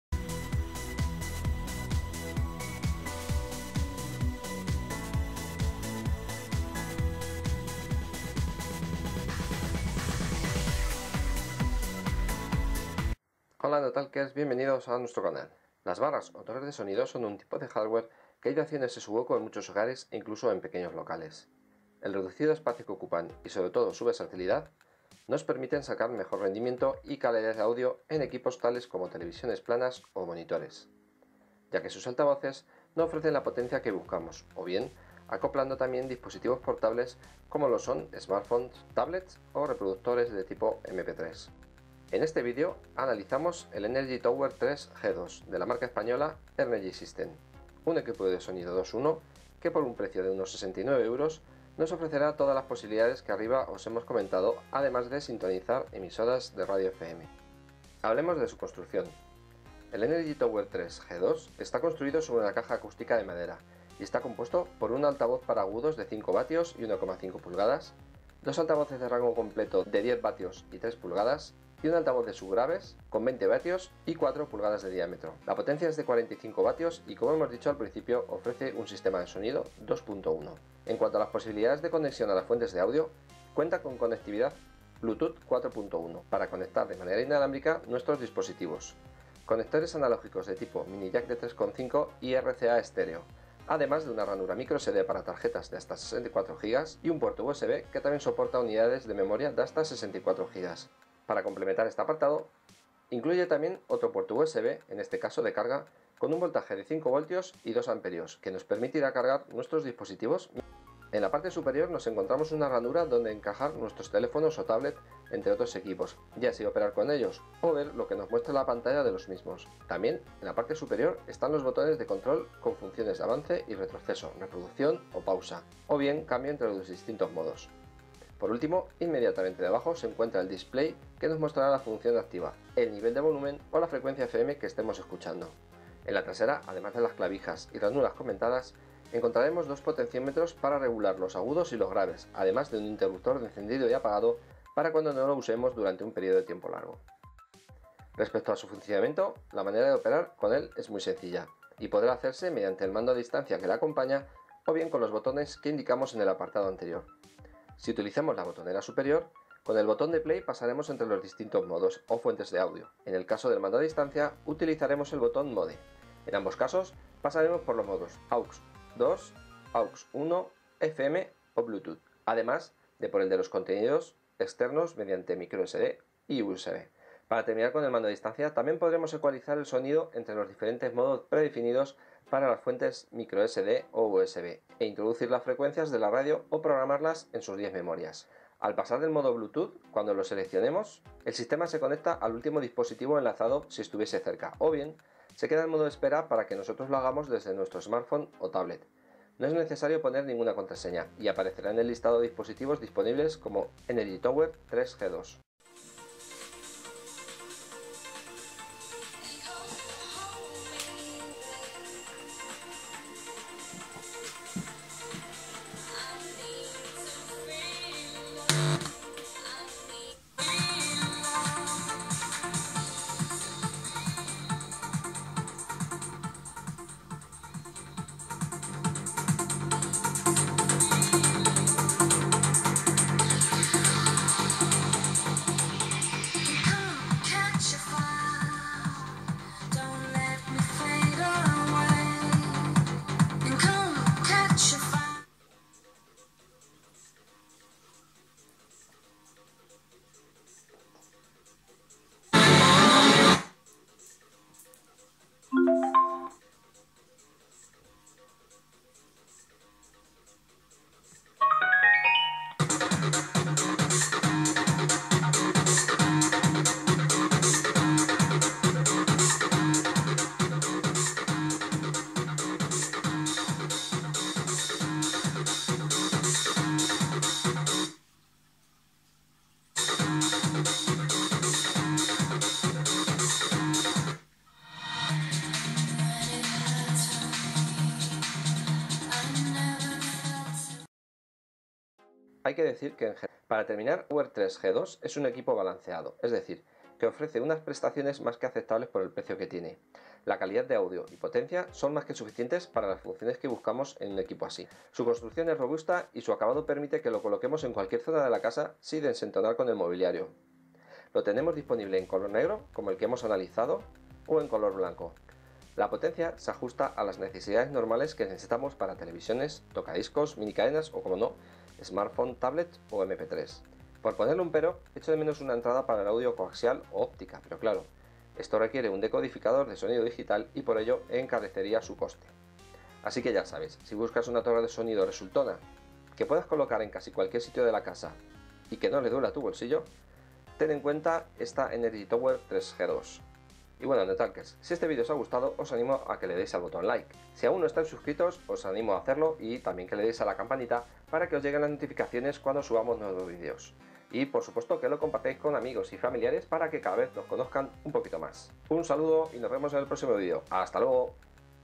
ENERGY TOWER 3 g2 | Análisis en Español
Adquiere aquí tu Energy Tower

Analizamos en este vídeo la torre de sonido la empresa Española Energy Sistem, la Energy Tower 3 G2. Es un sistema de sonido 2.1 con 45W de potencia, varios altavoces y con unas características interesantes además de un precio de entre 60€ para su versión en Negro y 69,90€ en la de color Blanco.